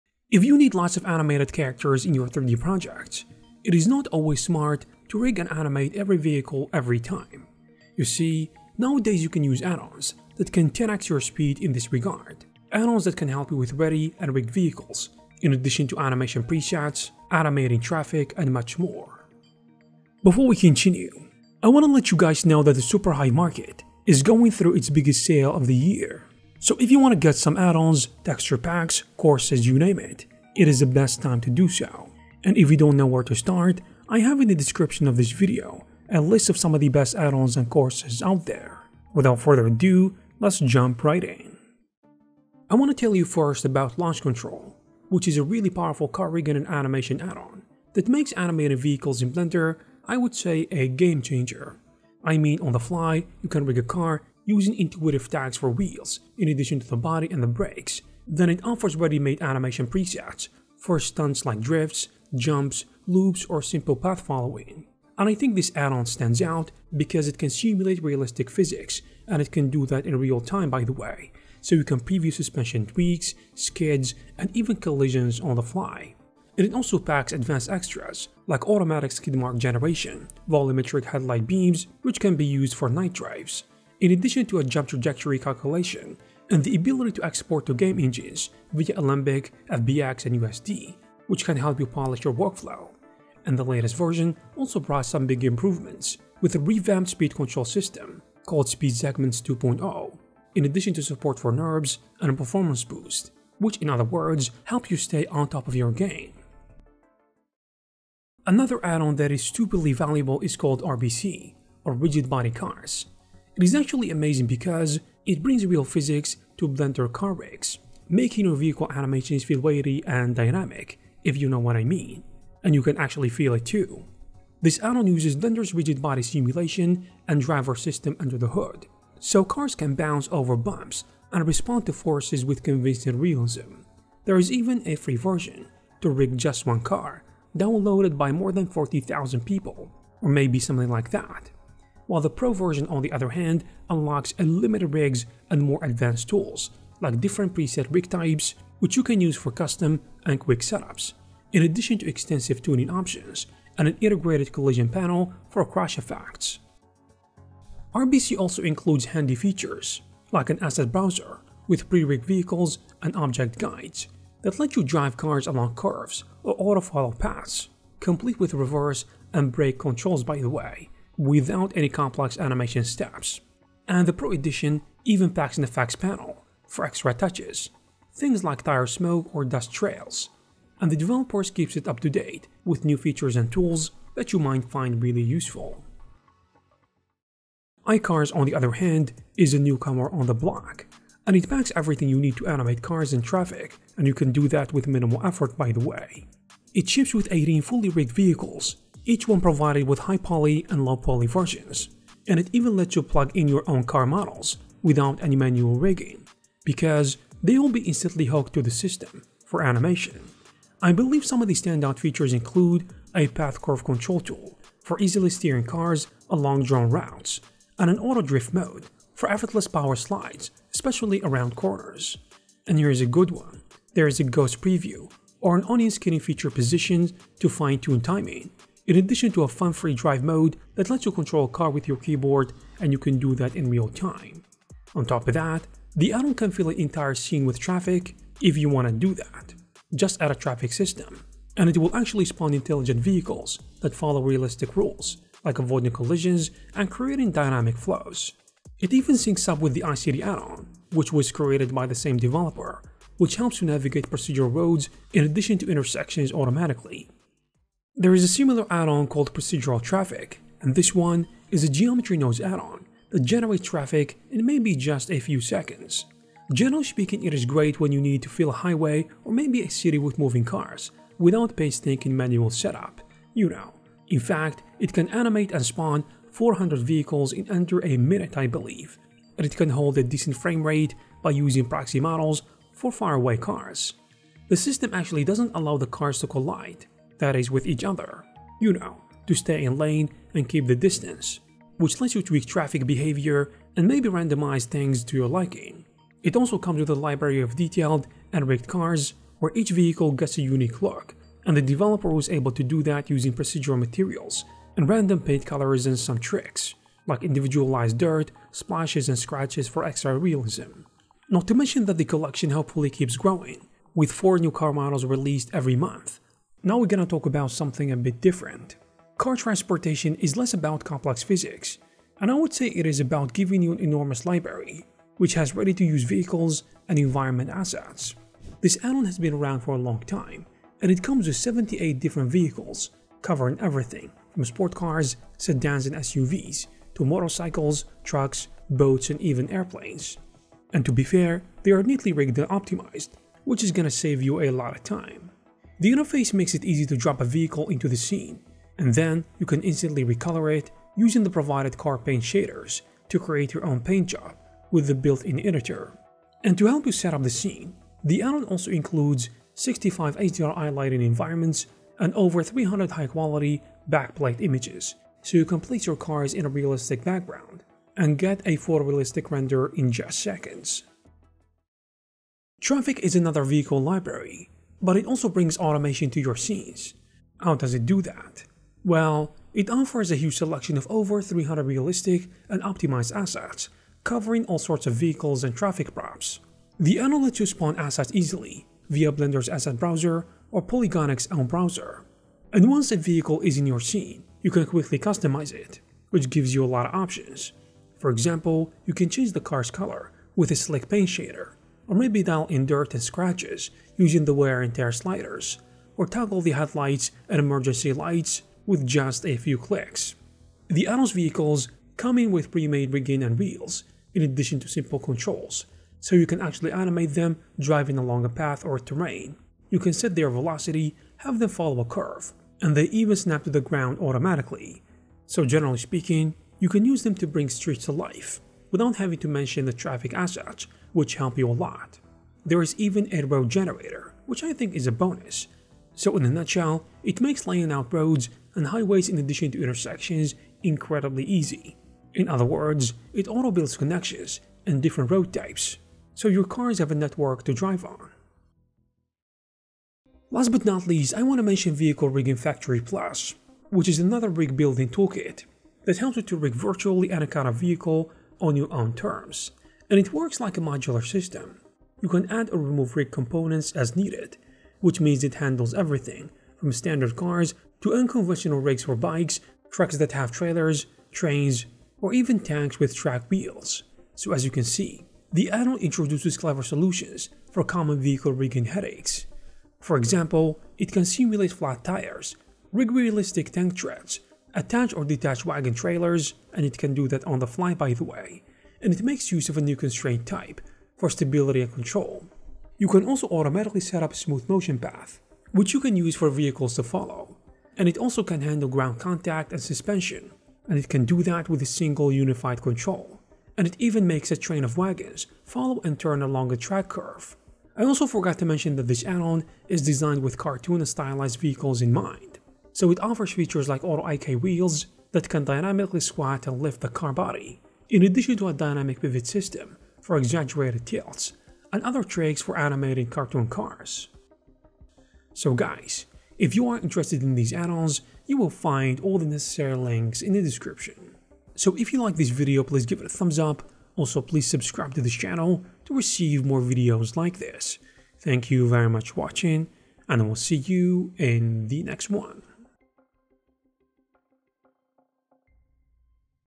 Blender Addons For Vehicle Rigging & Animation
00:00 Intro

00:42 Launch Control

01:47 Rbc

03:07 Icars

04:18 Procedural Traffic

05:16 Car Transportation

06:11 Car Library

07:30 Vehicle Rig Factory Plus

If you need lots of animated cars in your 3D projects, it is not always smart to rig & animate every vehicle every time; nowadays, you can use addons that can 10X your speed.

Productivity►:
Clean Panels